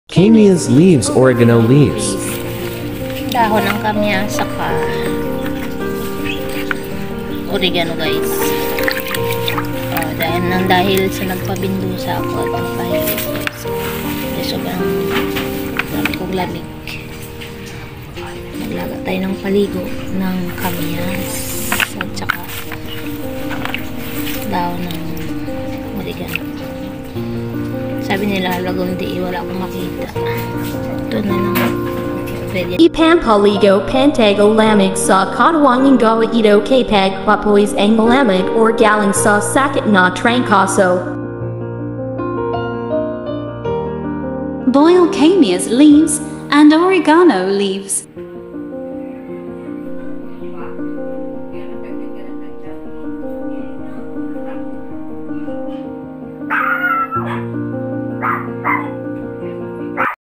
kamias leaves and oregano leaves
pampaligo pantanggal lamig sa katawan..
magpakulo ng dahon ng kamias at oregano sa 2liters na tubig .after kulo palamigin hanggang maligamgam n lng tsaka iligo.
gamot ito sa pantanggal lamig ng katawan
at mga galing sakit like trangkaso.